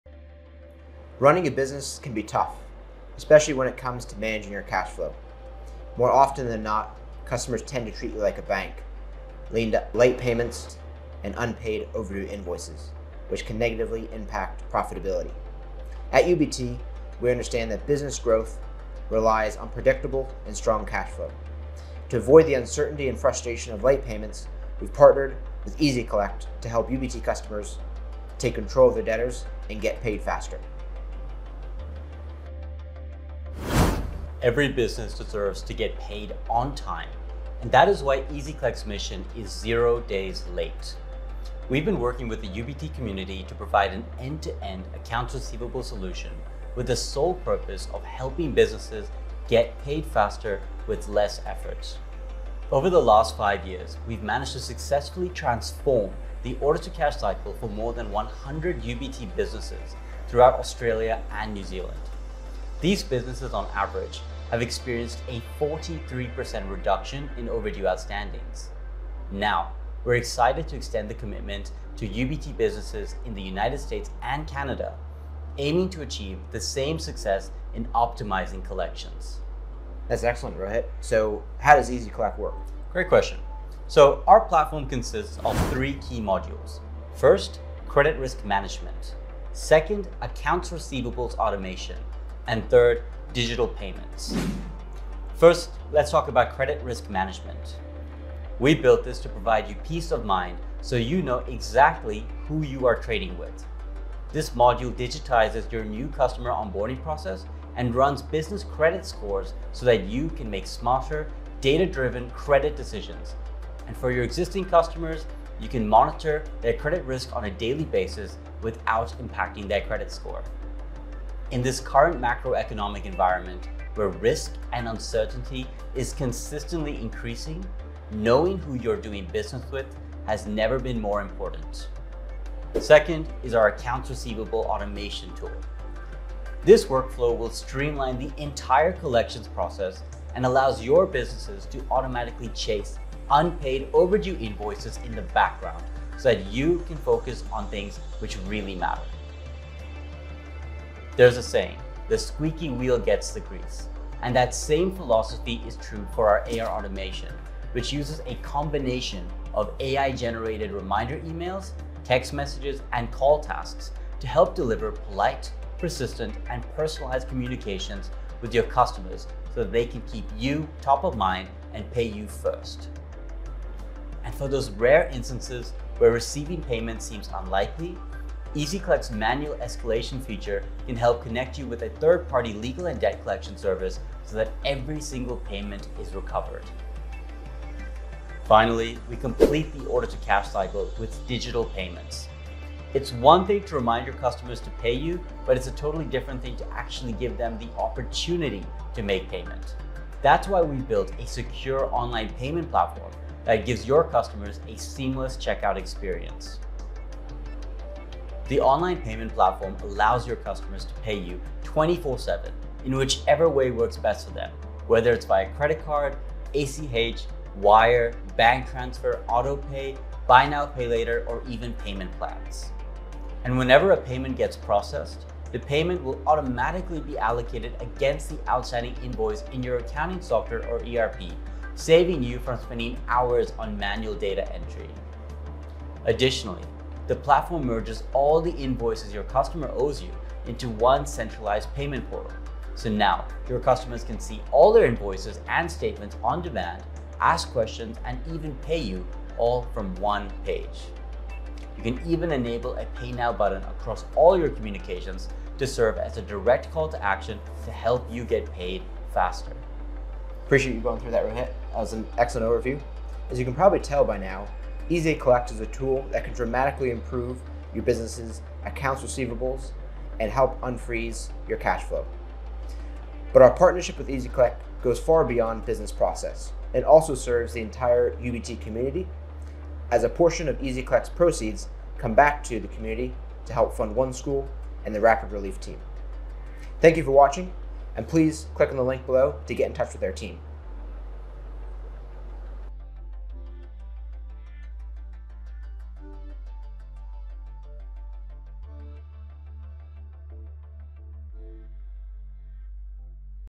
UBT North America and ezyCollect Partnership - Boost Your Cash Flow
Running a business can be tough, especially when it comes to managing cash flow. Late payments and overdue invoices can hurt your profitability. The experts at UBT know that your business growth relies on predictable and strong cash flow. That's why they've teamed up with ezyCollect to help you take control of your debtors and get paid faster.

Success in Australia and New Zealand
Over the past 5 years, ezyCollect has transformed the order-to-cash cycle for over 100 UBT businesses in Australia and New Zealand, leading to a 43% reduction in overdue invoices. Now, we’re excited to bring this success to UBT businesses in the United States and Canada.

How ezyCollect Works
Our platform has three key modules designed to make your life easier:
1. Credit Risk Management: Easily onboard new customers, run business credit scores, and monitor credit risk daily.
2. Accounts Receivable Automation: Automate your collections process with AI-generated reminders and manual escalation to legal and debt collection services.
3. Digital Payments: Give your customers a secure, seamless way to pay you online, with various payment options and automatic payment allocation.

Community Support
We're proud that a portion of ezyCollect's proceeds supports the UBT community, including funding for a school and the Rapid Relief Team.